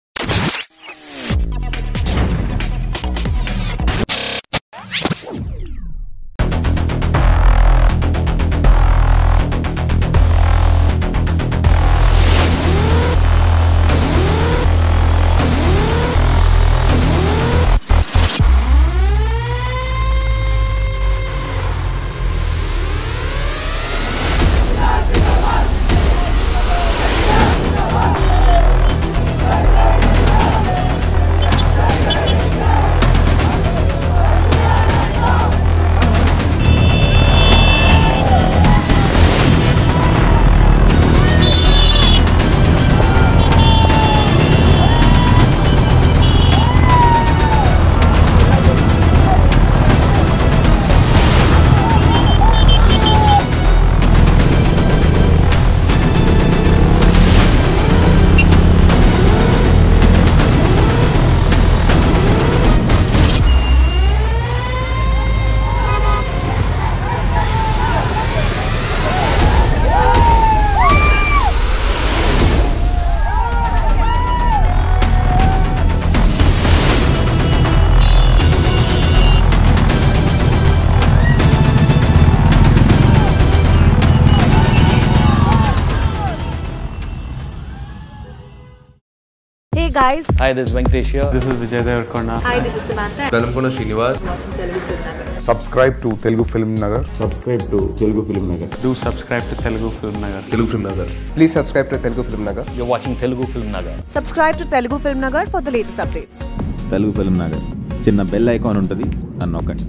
Jr NTR Fans Mass Rally | Jr NTR Karnataka Fans Hungama | RRR Pre Release Event At Chikkaballapur
Jr NTR Fans Mass Rally. Watch Jr NTR Karnataka Fans Hungama at RRR Pre Release Event at Chikkaballapur. RRR​ 2022 Indian Multi Lingual Movie ft. Jr NTR as Komaram Bheem, Ram Charan as Alluri Sitarama Raju, Alia Bhatt, Olivia Morris, Ajay Devgn and Shriya Saran. Directed by Rajamouli. Produced By DVV Danayya under DVV Entertainments. Music by MM Keeravani. JrNTR​ RamCharan RRRMovie NTR SSRajamouli AliaBhatt​